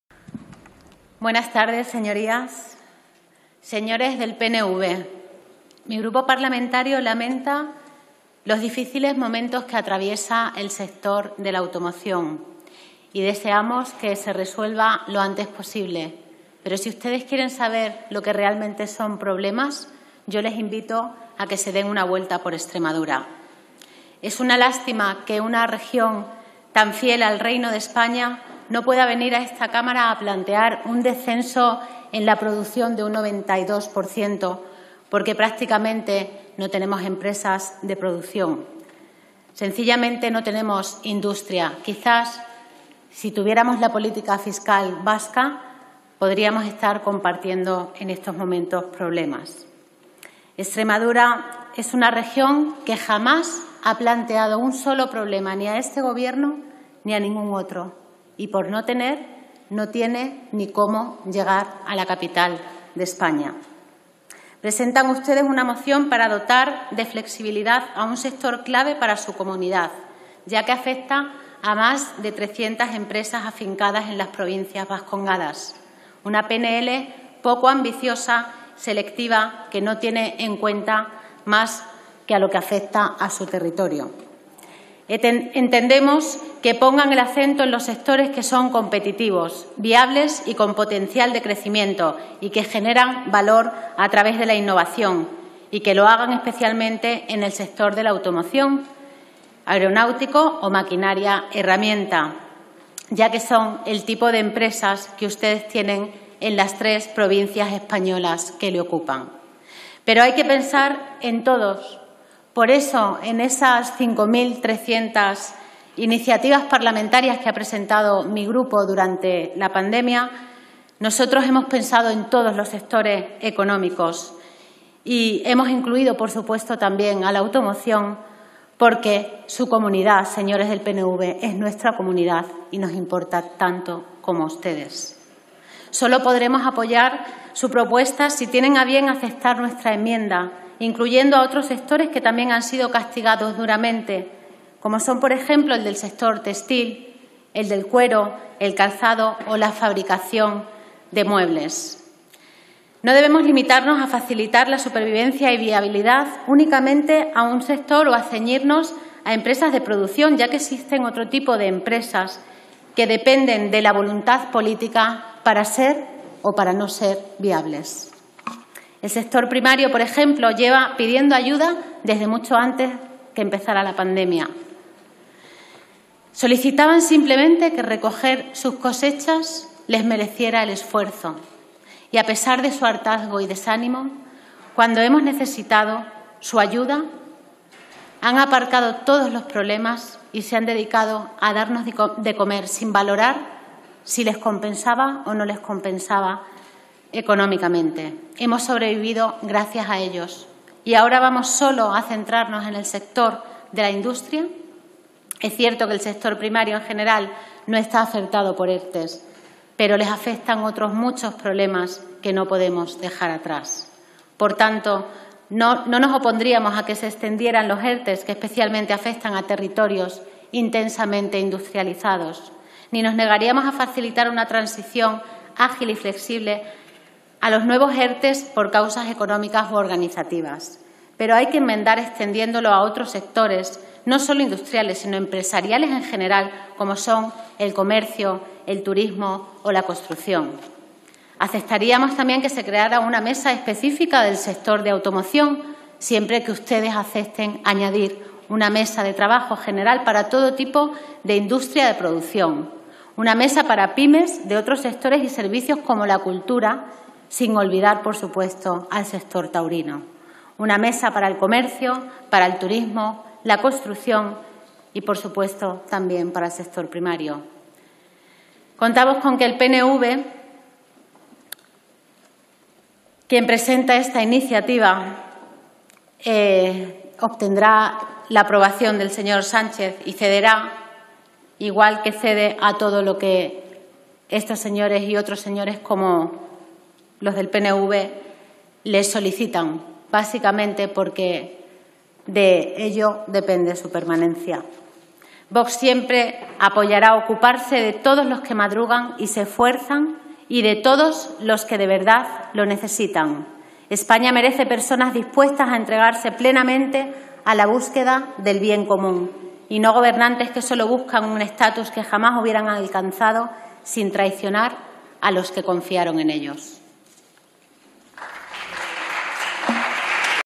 Intervención Nevado del Campo sobre flexibilidad a los sectores clave. 16 de Junio de 2020
Intervención María Magdalena Nevado del Campo de VOX, sobre las medidas previstas para dotar de flexibilidad a los sectores clave de la actividad económica, el día 16 de Junio de 2020. Recuperado de